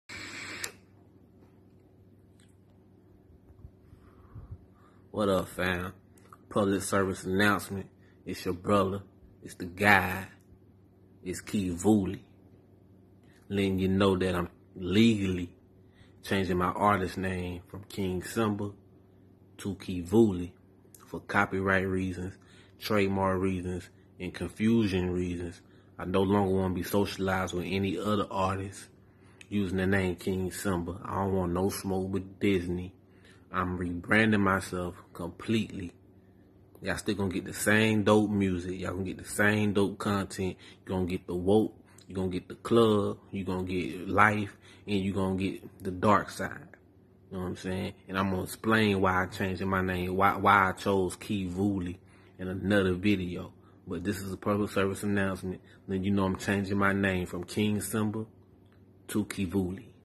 Why I Rebranded Myself...Pt.1
*REBRANDING*

I HAVE OFFICIALLY (Legally) CHANGED MY ARTIST NAME FROM - KING SIMBA TO KIVU7I (Kivuli)

Public Announcement and Reasoning  given in these videos!

Label: @heruimperiumnetwork
Manager: @driven_destined